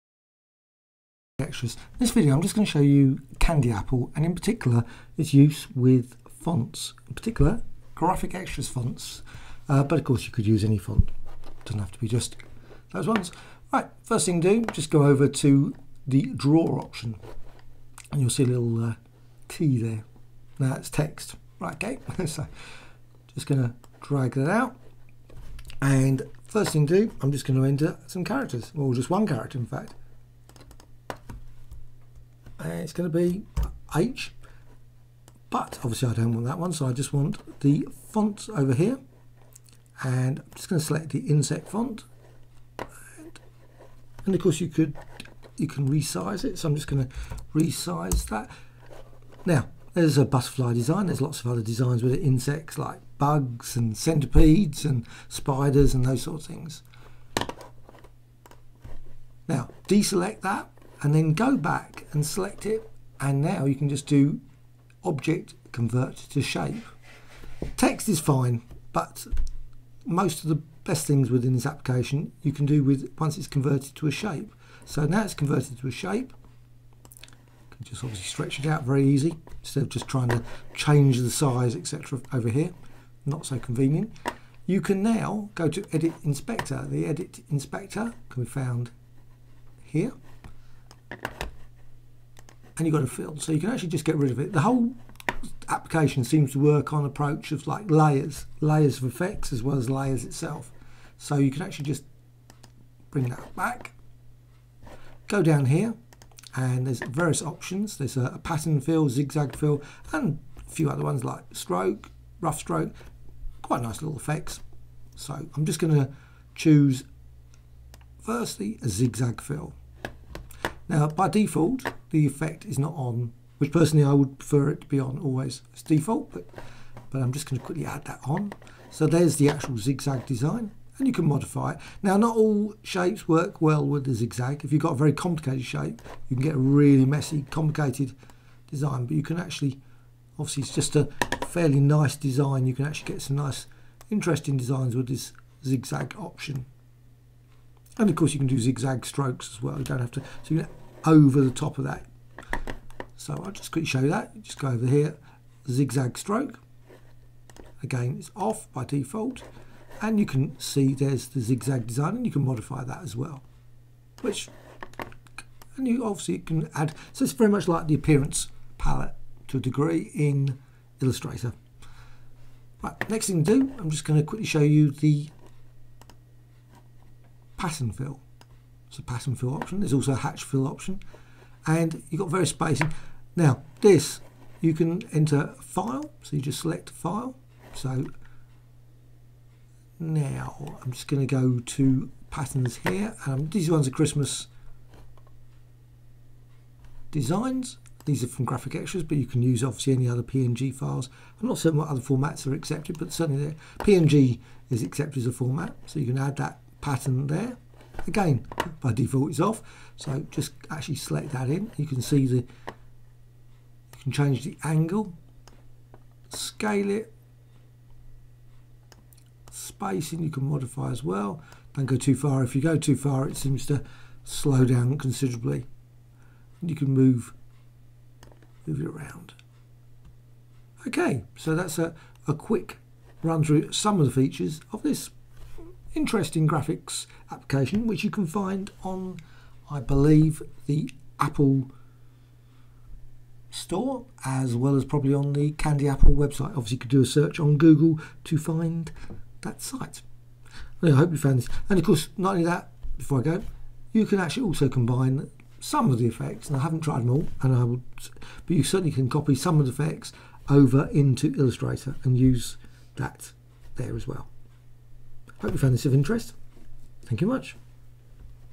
Candy apple and text vector designs and zigzags and patterns tutorial
The Candy apple vector software can be used to add some interesting effects to designs such as fonts / type via the text tool (such as the insect font set)